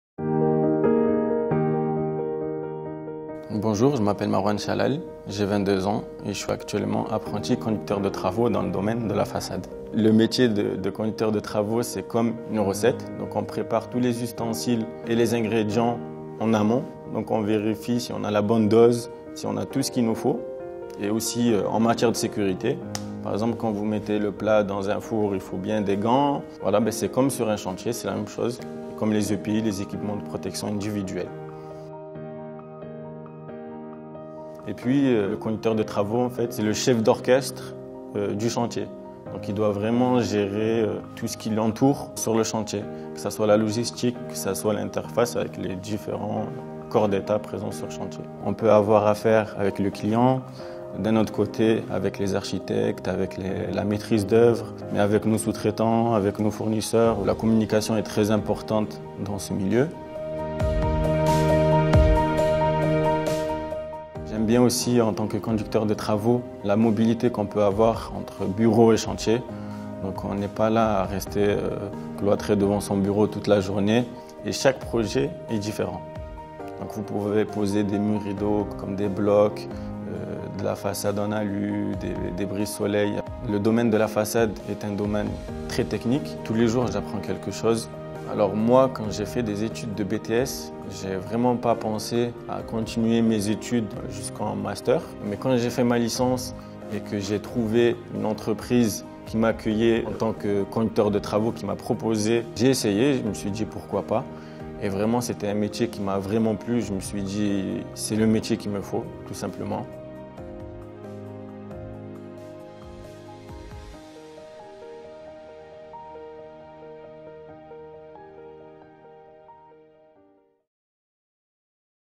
Métiers Menuiserie aluminium - Conducteur de travaux
Découvrez  Marouane, conducteur de travaux chez Permasteelisa, passionné par son métier qu'il exerce en région parisienne.

En tant que conducteur de travaux, il prépare et pilote l’intervention sur les chantiers pour optimiser la coordination des équipes. Un métier excitant et à forte responsabilité qui attire des profils audacieux et rigoureux.

Ce qu'il lui plait dans son métier ? La mobilité entre bureau et chantier et de travailler sur des projets différents.

Si toi aussi, tu veux devenir conducteur de travaux dans la menuiserie aluminium ?